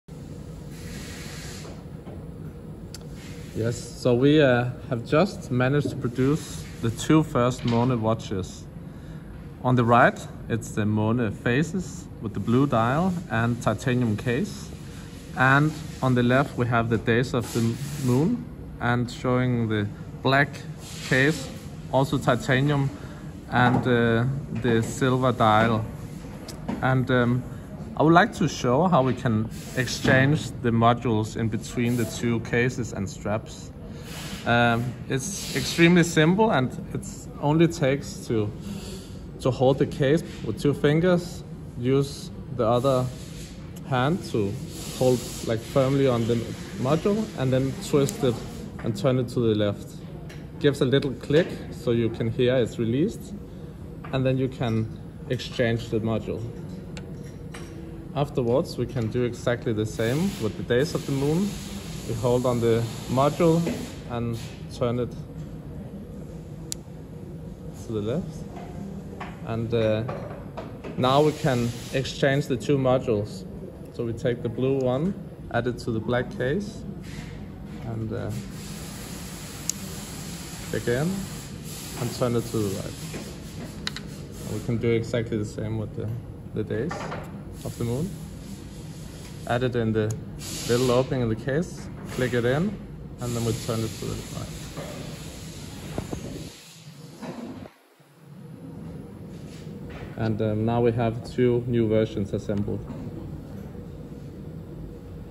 The Funder Måne prototypes - the modularity
Thomas Funder explains the modularity of his watch and how you  can exchange the module and case.

The Måne is currently in small batch production after having been funded in the Angel round.